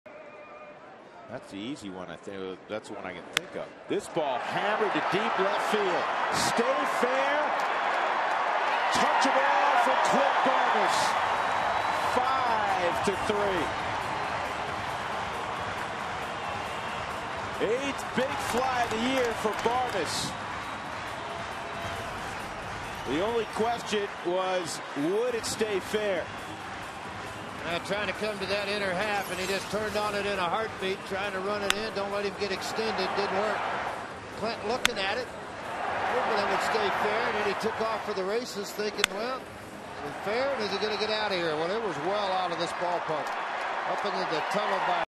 2009/06/21 Barmes' solo blast
PIT@COL: Barmes skies a homer to left in the fourth